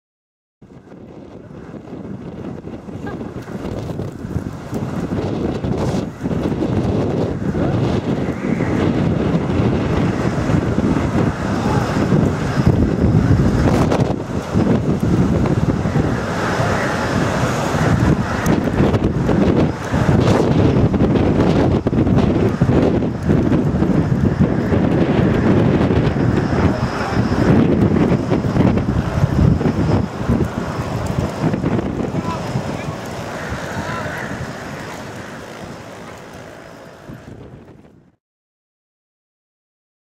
Trotseer wind bij faculteit EWI @TU Delft
Scholieren trotseren windkracht 7 om bij Wiskunde Actief te komen